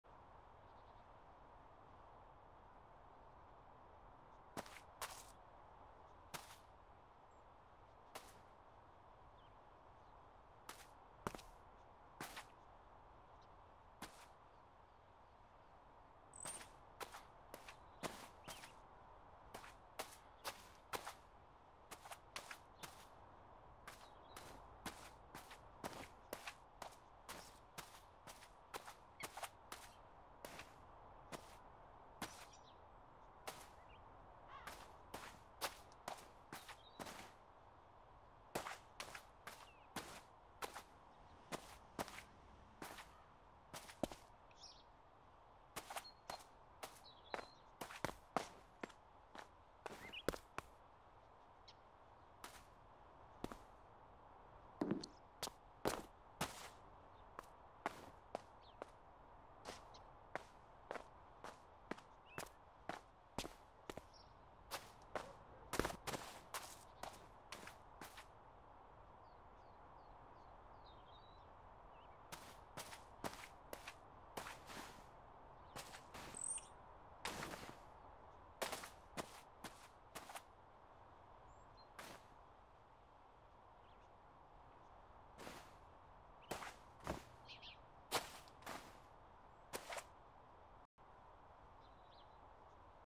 why you don't eat taco bell in your backyard 🤦‍♂️
don't give your gardener or anybody in your backyard taco bell because it'll end up looking like this Mexican spice battlefield right here